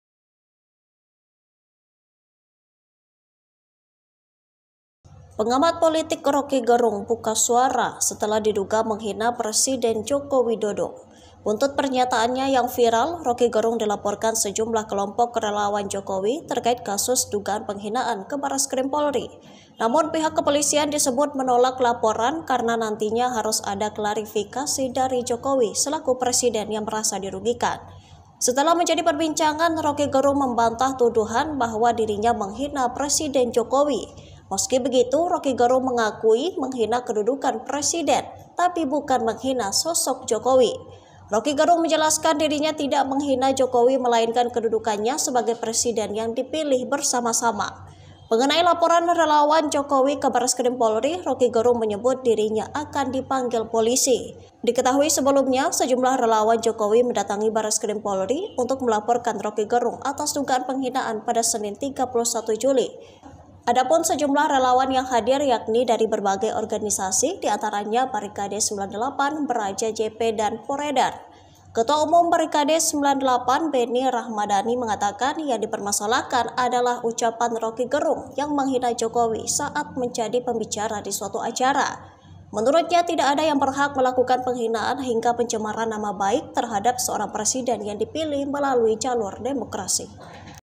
Rocky Gerung Bantah Hina Presiden Joko Widodo
TRIBUN-VIDEO.COM - Pengamat politik Rocky Gerung buka suara setelah diduga menghina Presiden Joko Widodo (Jokowi).

Buntut pernyataannya yang viral, Rocky Gerung dilaporkan sejumlah kelompok relawan Jokowi terkait kasus dugaan penghinaan ke Bareskrim Polri.

Namun, pihak kepolisian disebut menolak laporan karena nantinya harus ada klarifikasi dari Jokowi selaku presiden yang merasa dirugikan.

Setelah menjadi perbincangan, Rocky Gerung membantah tuduhan bahwa dirinya menghina Presiden Jokowi.

Meski begitu, Rocky Gerung mengakui menghina kedudukan presiden, tapi bukan menghina sosok Jokowi

Rocky Gerung menjelaskan dirinya tidak menghina Jokowi melainkan kedudukannya sebagai presiden yang dipilih bersama-sama

Mengenai laporan relawan Jokowi ke Bareskrim Polri, Rocky Gerung menyebut dirinya akan dipanggil polisi.

Diketahui, sejumlah relawan Jokowi mendatangi Bareskrim Polri untuk melaporkan Rocky Gerung atas dugaan penghinaan, Senin (31/7/2023).

Adapun sejumlah relawan yang hadir yakni dari berbagai organisasi di antaranya Barikade 98, Bara JP, dan Poreder.

Ketua Umum Barikade 98, Benny Rhamdani, mengatakan yang dipermasalahkan adalah ucapan Rocky Gerung yang menghina Jokowi saat menjadi pembicara di suatu acara.

Menurutnya, tidak ada yang berhak melakukan penghinaan hingga pencemaran nama baik terhadap seorang presiden yang dipilih melalui jalur demokrasi.